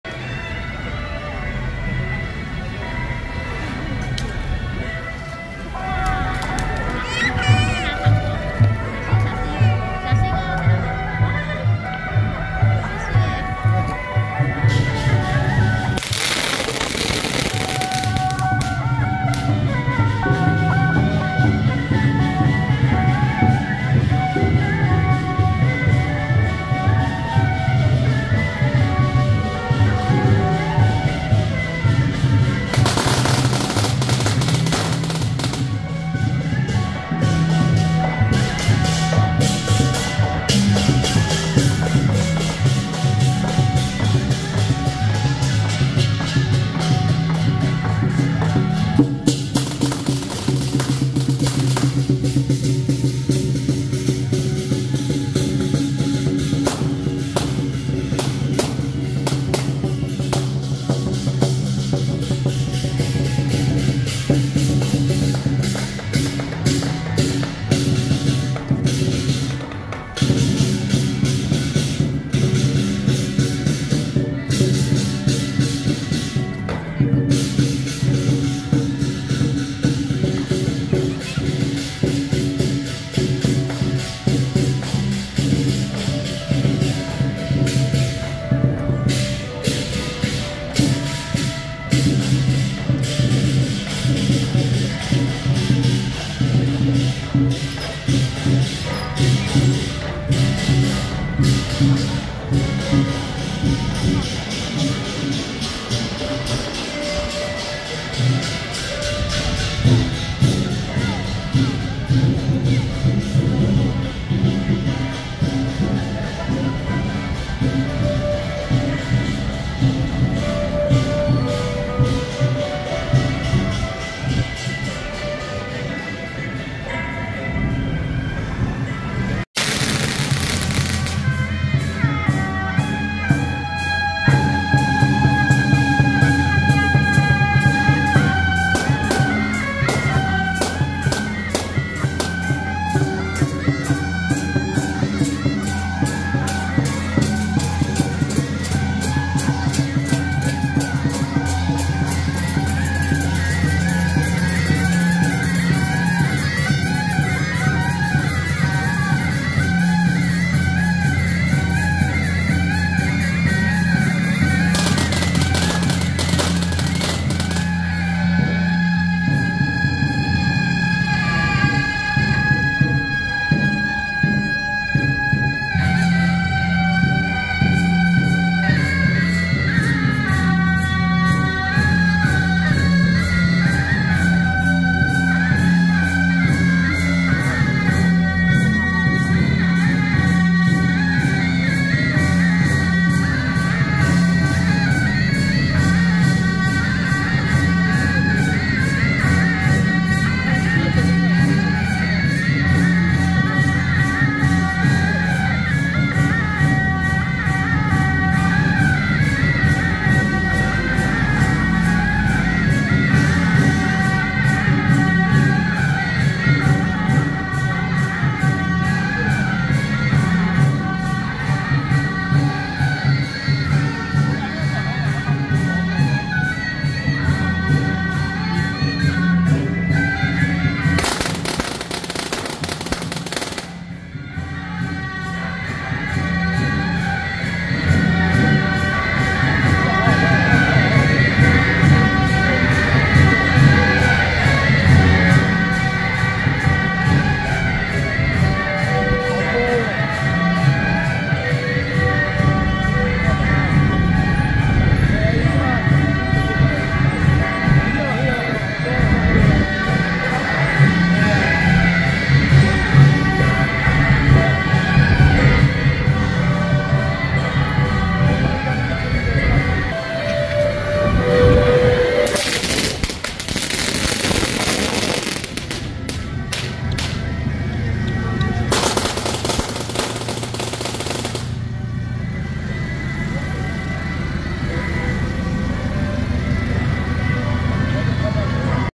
【新莊玄合宮 慶贊 2023新北市玄天上帝文化祭】~2023新北市玄天上帝文化祭三重地區祈福遶境
(0:00)新莊玄合宮-頭旗花車,鼓亭車(棟發)
(0:37)台北祥薪舘-技術龍
(1:26)新莊玄合宮-花牌車
(2:12)三峽聖福堂-大鼓弄＆峽水神將會+新莊玄合宮-神將團
(4:31)新莊玄合宮-北極玄天上帝 聖駕